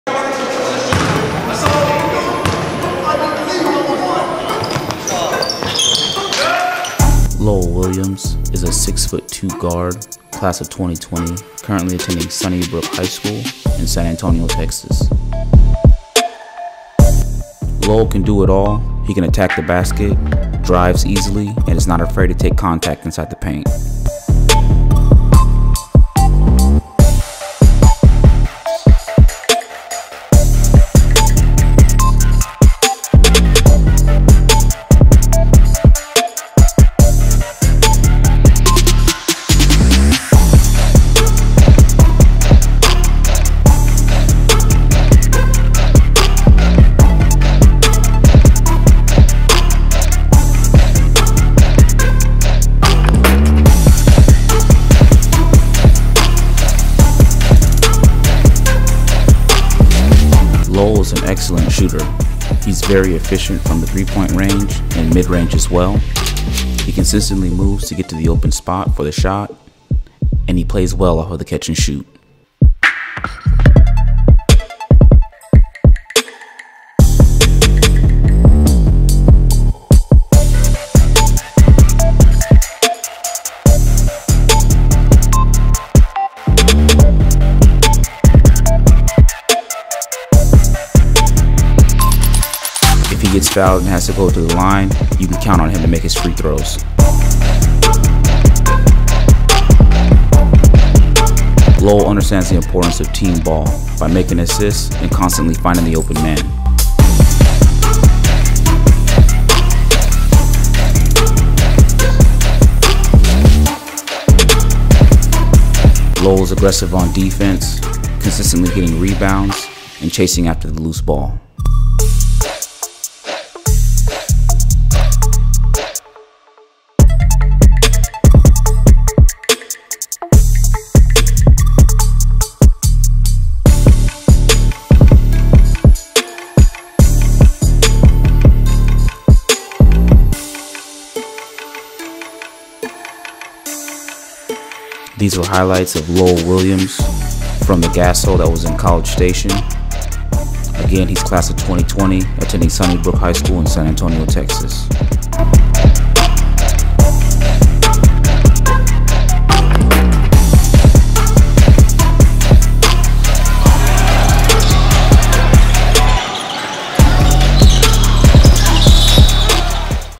Lowell Williams Scouting Video: GASO Bryan/College Station (2019)
Scouting video of Lowell Williams (Sunnybrook Christian HS - c/o 2020).

These highlights are from Lowell's performance at the Great American Shootout (GASO) that was Bryan/College Station (June 2019).

Lowell Williams
6'2" G/SG

Scouting video produced by Texas Basketball Scouts.